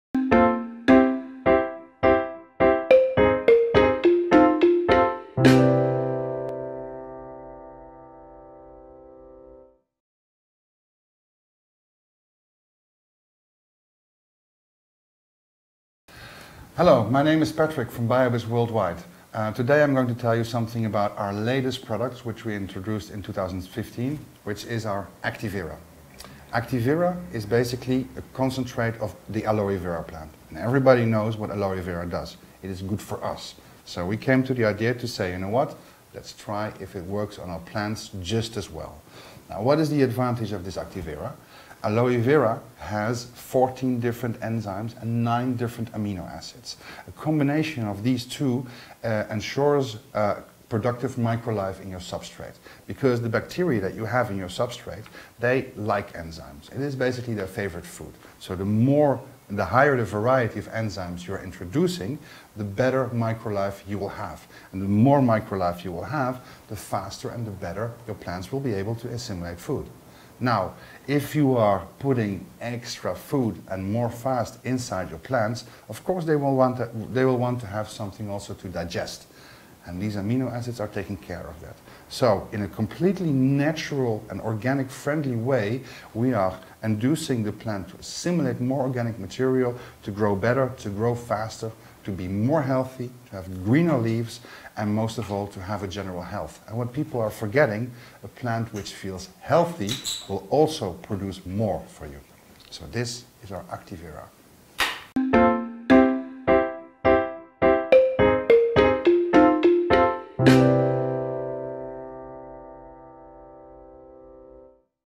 Learn how Activera works from Mr Bloom - BioBizz in South Africa
Patrick Stevens aka Mr Bloom teaches us about Activera from BioBizz while visiting South Africa.

Activera is the latest product from BioBizz World Wide Organics.
It promotes a productive micro-life in your grow medium. Containing 14 different enzymes & 9 different amino acids you can rest assured your plants have sufficient food. The better your micro-life the faster your plants will be able to assimilate the nutrition supplied.

Buy Online:
BioBizz Try Pack
BioBizz Root-Juice
BioBizz Fish-Mix
BioBizz Bio-Heaven
BioBizz Acti-Vera
BioBizz Bio-Grow
BioBizz Bio-Bloom
BioBizz Top-Max
BioBizz Alg-A-Mic

Transcription:

Hello. My name is Patrick from BioBizz Worldwide.
Today I`m going to tell you something about our latest product, which we introduced in 2015, which is Activera. Activera is basically a concentrate of the aloe-vera plant. And everybody knows what aloe-vera does - it is good for us. So we came to the idea to say: “You know what? Let’s try if it works on our plants just as well.” What is the advantage of this Activera?
Aloe-vera has 14 different enzymes, 9 different amino acids. Combination of this two ensures productive microlife in your substrate, because the bacteria that you have in your substrate, they like enzymes. It is basically their favorite food. So, the more, the higher the variety of enzymes you introducing, the better microlife you will have. And the more microlife you will have, the faster and the better your plants will be able to assimilate food.
Now, if you are putting extra food and more fast inside your plants, of course they will want to have something also to digest. And these amino acids are taking care of that.
So, in a completely natural and organic friendly way, we are inducing the plant to assimilate more organic material to grow better, to grow faster, to be more healthy, to have greener leafs, and most of all to have general health. People are forgetting a plant which feels healthy, will also produce more for you.
So this is our Activera.

Привіт. Мене звати Патрік і я із BioBizz Worldwide.
Сьогодні я розкажу вам дещо про наш останній продукт, який ми презентували у 2015 році - Activera. Activera - це, власне, концентрат із алое-вера. А кожен знає, що робить алое-вера - воно дуже корисне для нас. Отже нам прийшла ідея сказати “А знаєте що? А давайте спробуємо його і на рослинах також.” То ж які переваги має Activera?
Алое-вера містить у собі 14 різних ензимів та 9 різних амінокислот. Комбінація цих двох елементів забезпечує продуктивне мікрожиття у вашому субстраті, через те, що бактерії, котрі ви маєте у своєму субстраті, люблять ензими. Це, власне, їхня улюблена їжа. Отже, чим більшу, чим ширшу різноманітність ензимів ви запропонуєте, тим краще мікрожиття ви отримаєте. А чим більше мікрожиття ви маєте, тим швидше і краще ваші рослини будуть поглинати їжу.
Тепер, якщо ви дуже швидко введете додаткову їжу до ваших рослин, звичайно, їм потрібно щось для перетравлення. І про це подбають саме амінокислоти.
Отже, цілком природнім і органічно дружнім шляхом, ми змушуємо рослину поглинати більше органічного матеріалу для того, щоб рости краще, швидше, бути більш здоровою,мати більш зелене листя і бути здоровою в цілому. Люди забувають, що рослина, яка почуває себе здоровою, є більш продуктивною і для вас.
Отже, це і є наша Activera.